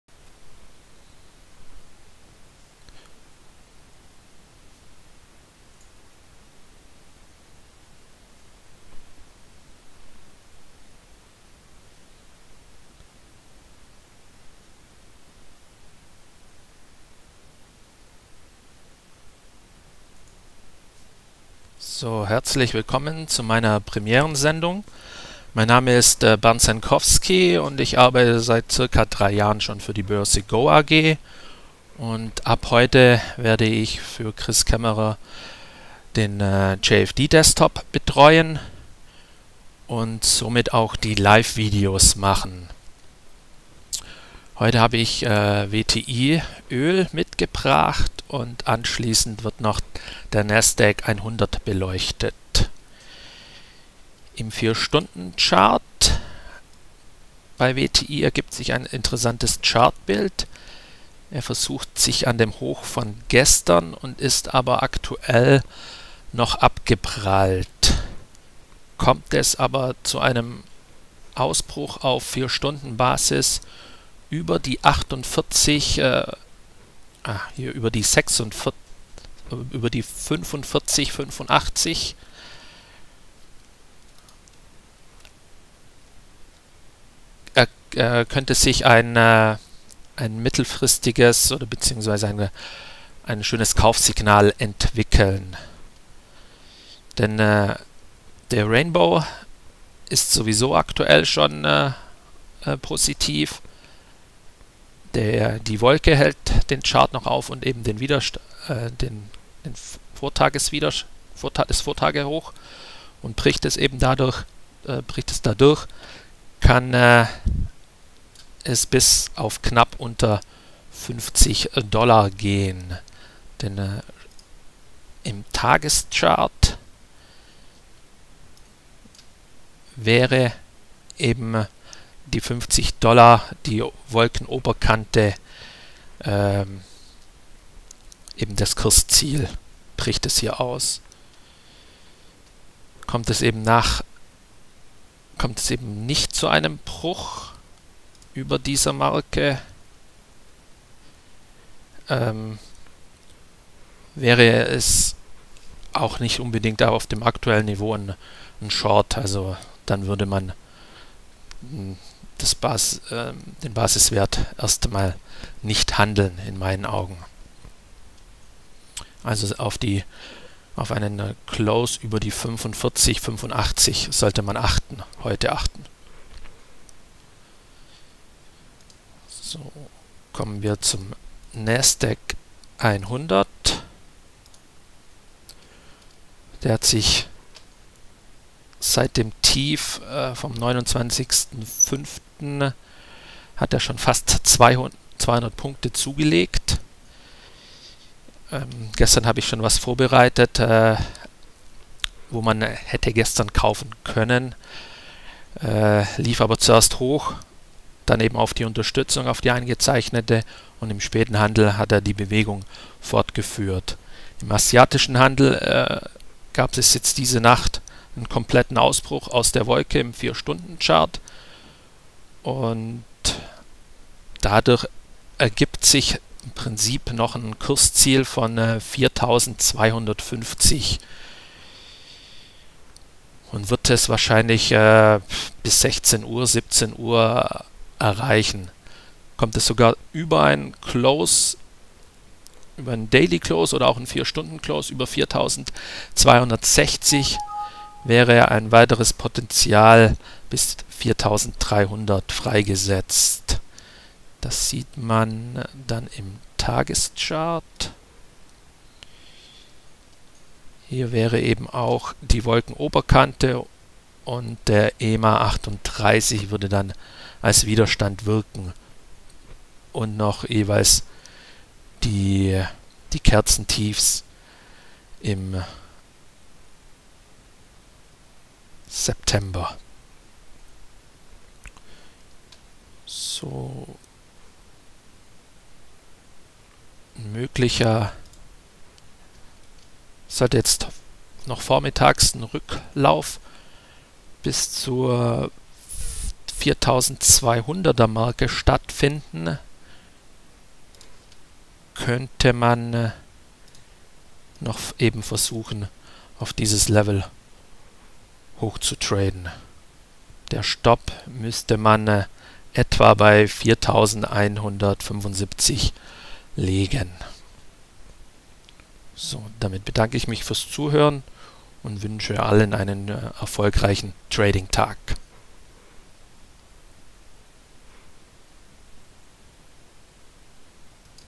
WTI ÖL am Buy-Trigger
Im 4H-Chart probt WTI ÖL den "Aufstand". Die Buy-Trigger 45,85 $ auf Schlusskursbasis ist 45,85 $. Zusätzlich wird kurz das Chartbild des Nasdaq 100 besprochen.